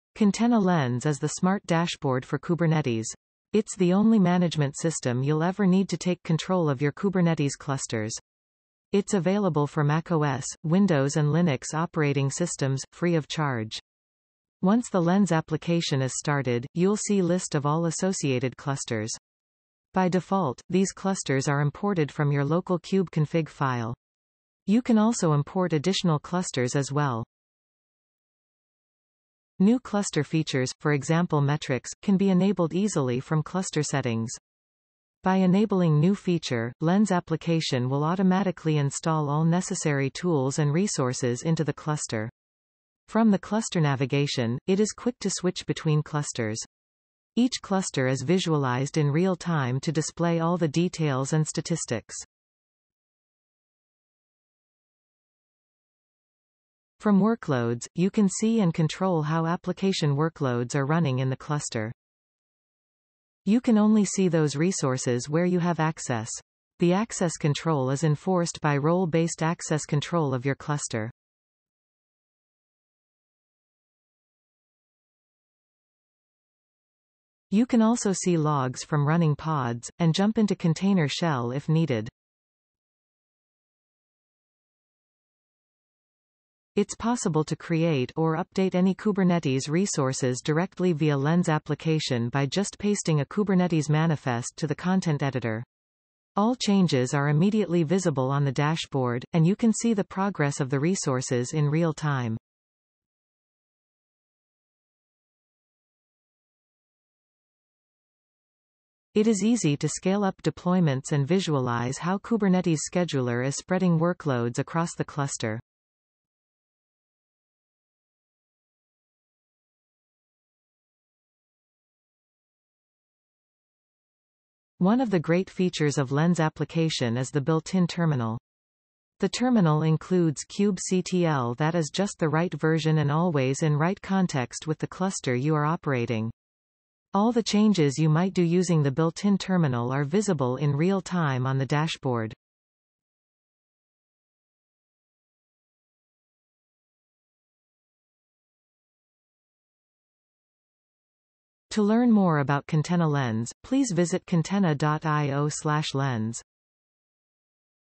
Kontena Lens - The Smart Dashboard for Kubernetes
Kontena Lens provides the most sophisticated user interface for managing Kubernetes clusters. It is the only management system you’ll ever need to take control of your clusters. Once you have tried it, there's no going back.